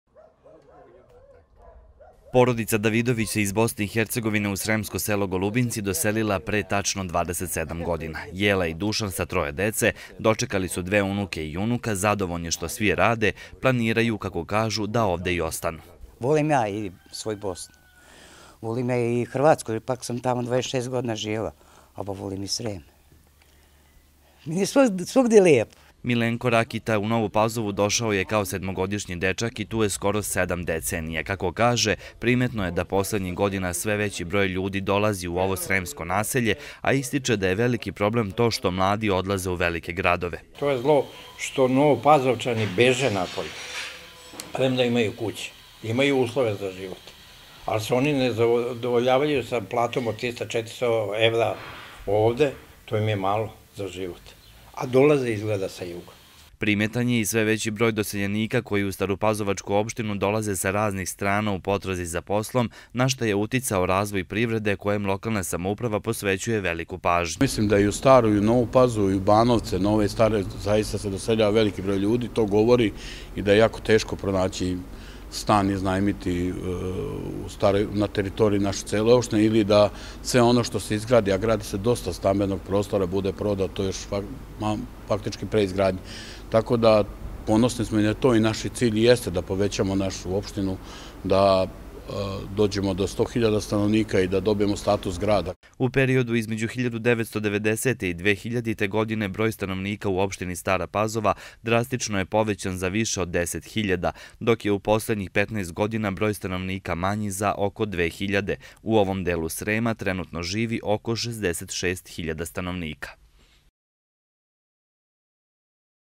Zašto ljudi dolaze u Staru Pazovu?
U jugoistočnom delu Srema trenutno živi blizu 66.000 stanovnika. Podaci poslednja dva popisa pokazuju da je broj stanovnika u opštini Stara Pazova drastično povećan između 1990 i 2000. godine što ukazuje da je baš u tom periodu primetno doseljavanje.